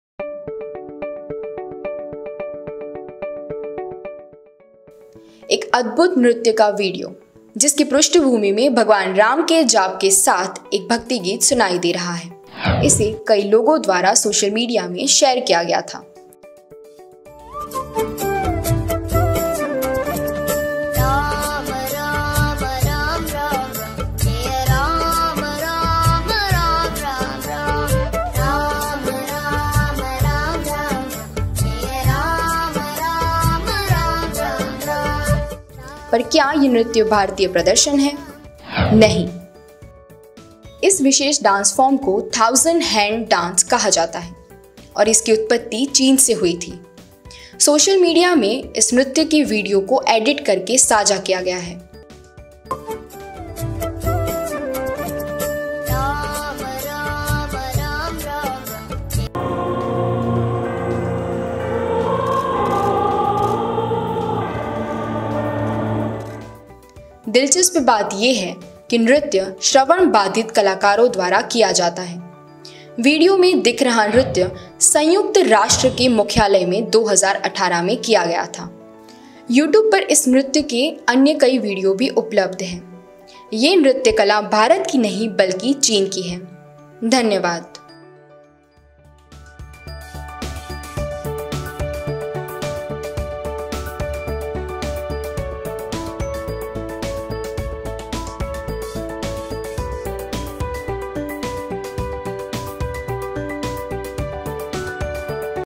चाइनीज़ डांस का वीडियो एडिट कर हिंदू भक्ति गीत पर प्रदर्शन के रूप में साझा
2018 में संयुक्त राष्ट्र मुख्यालय में एक चाइनीज़ डांस फॉर्म ‘Thousand hands dance’ के प्रदर्शन का वीडियो एडिट कर हिंदू भक्ति गीत पर भारतीय प्रदर्शन के रूप में सोशल मीडिया पर साझा किया गया।